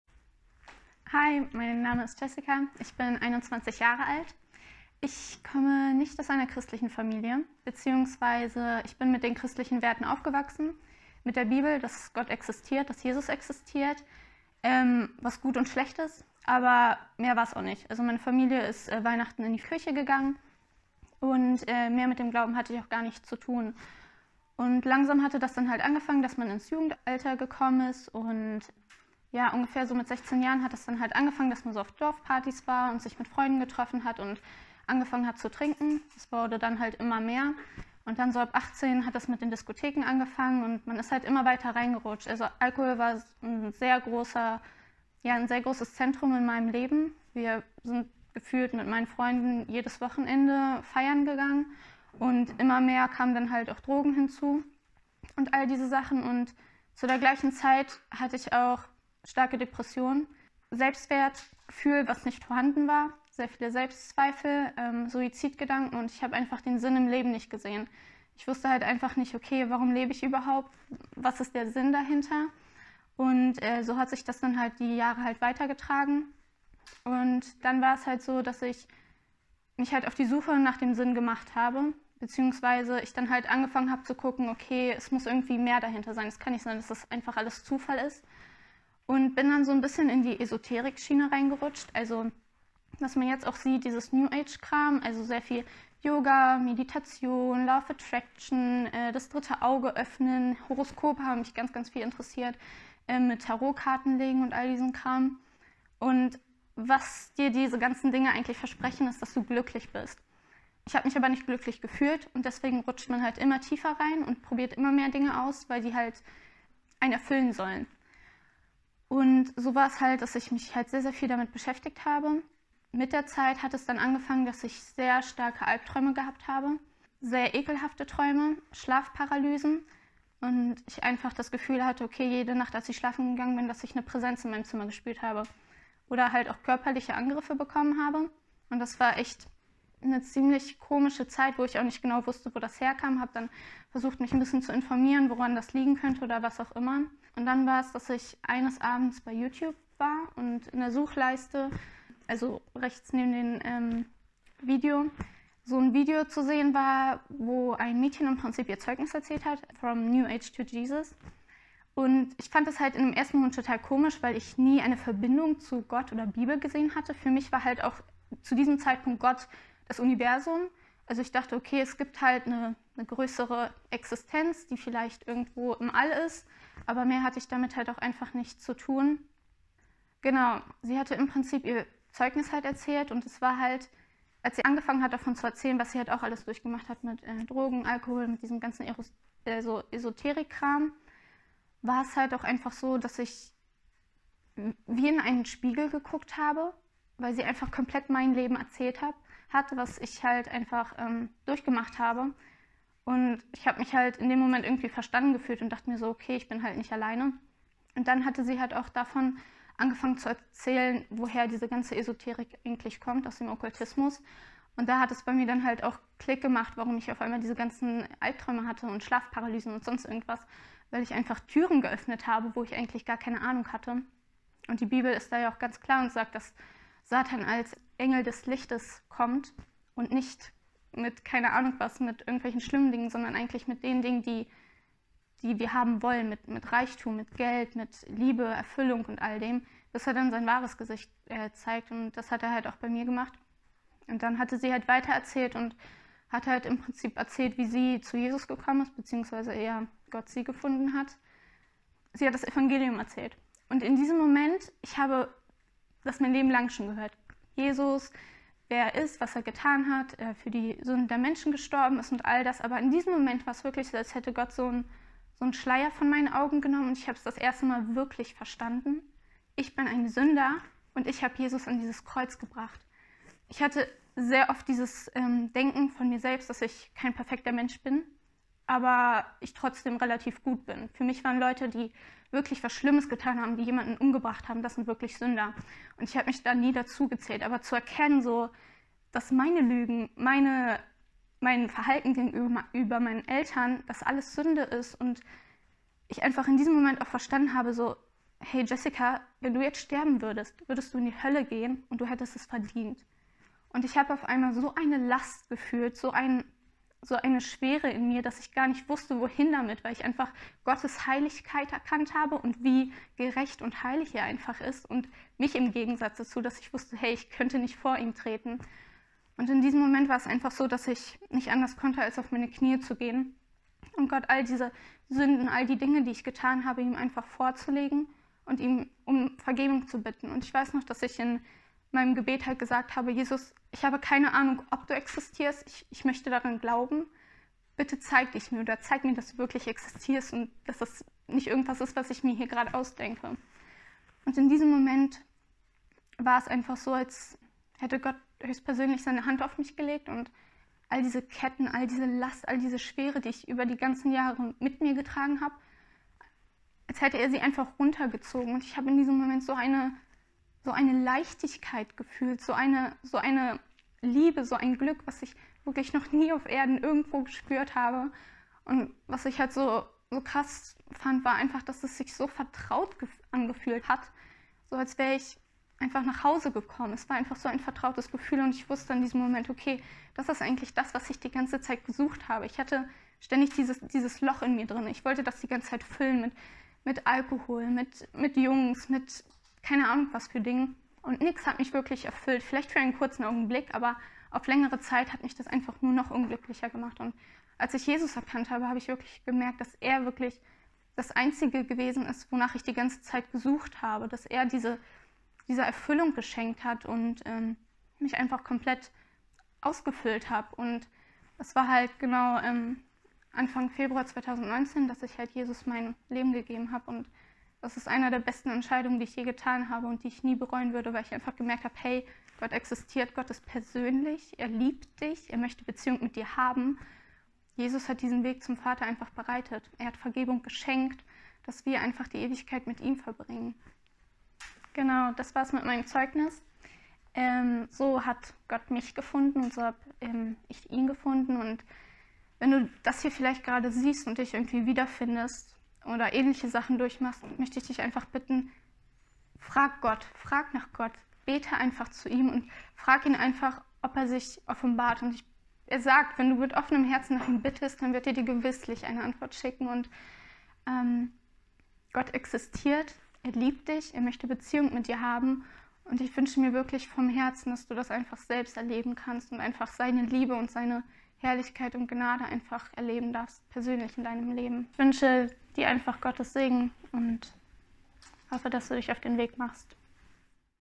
Jessica findet Jesus: Von Esoterik und Depressionen zu einem Leben in Fülle
Wenn ihr noch Fragen habt, dann könnt ihr, die gerne in die Kommentarbox stellen und wir werden die dann versuchen, zu beantworten.